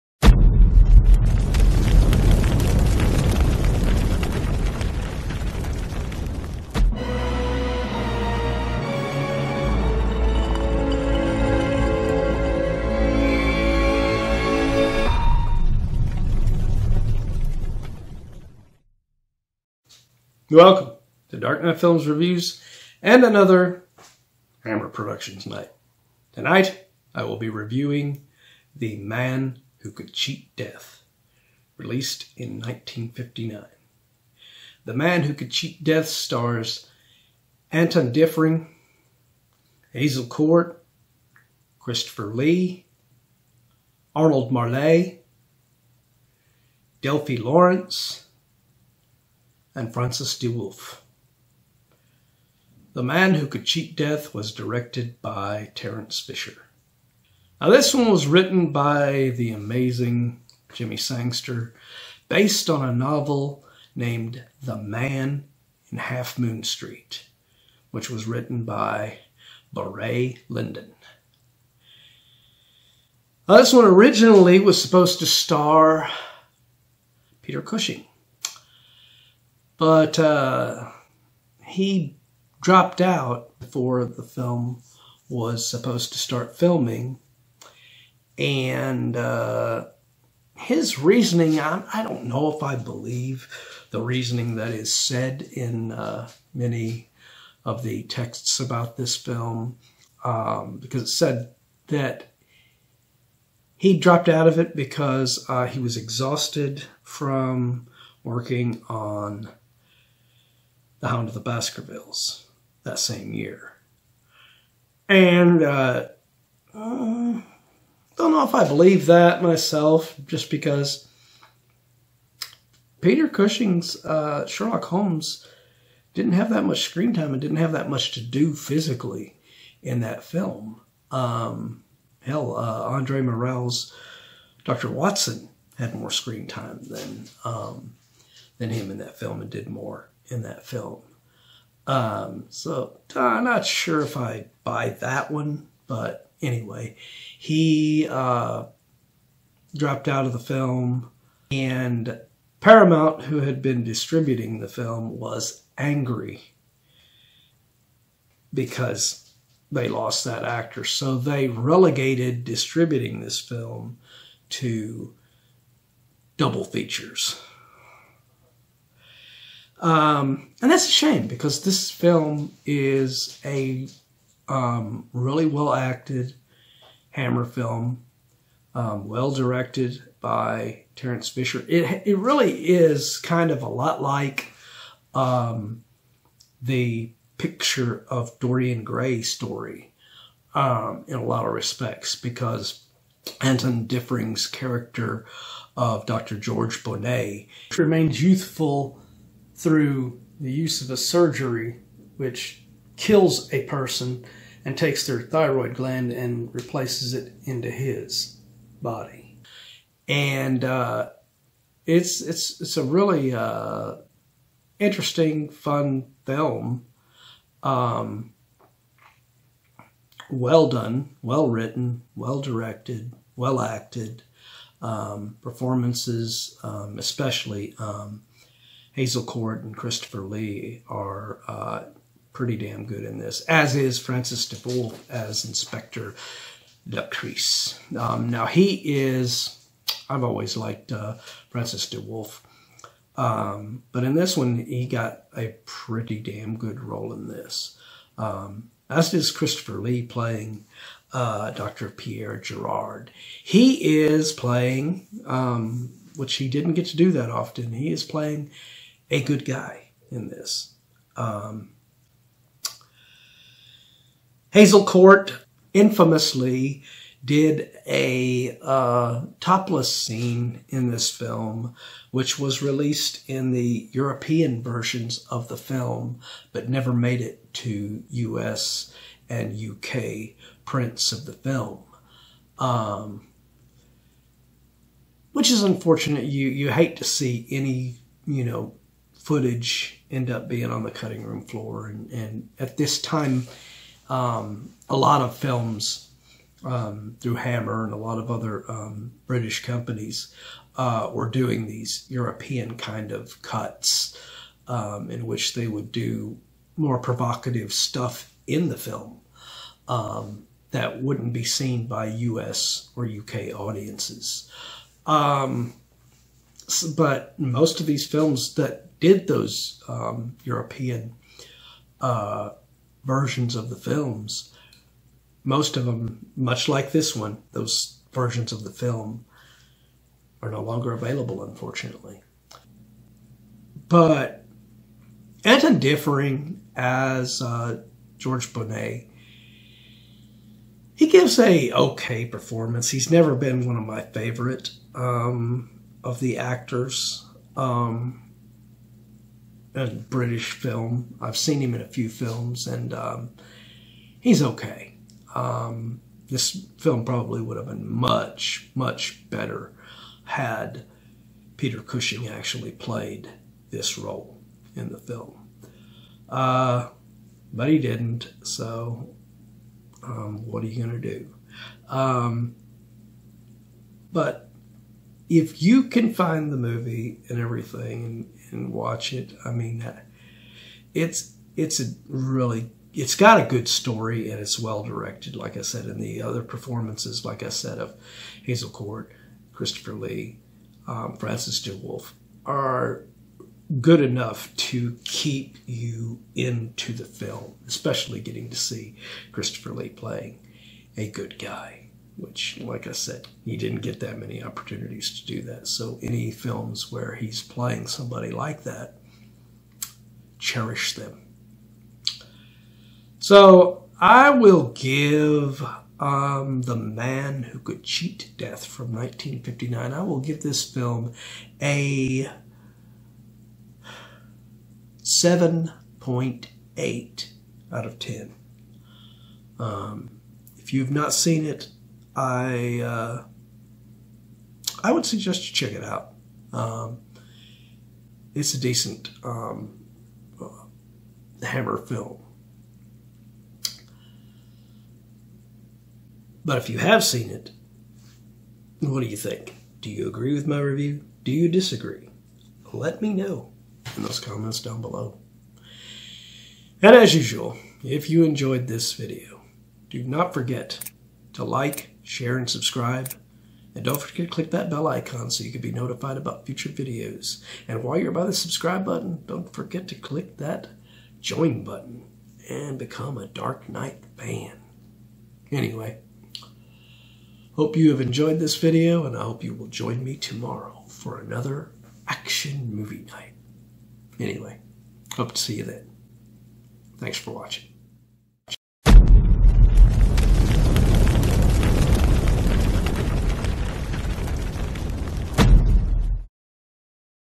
The Man Who Could Cheat Death (1959) Review
Dark Night Films host Matt Spease reviews the classic Hammer Film Productions horror film The Man Who Could Cheat Death (1959), directed by
Terence Fisher and starring Anton Diffring, Hazel Court, Christopher Lee, Arnold Marle, Delphi Lawrence, and Francis de Wolff.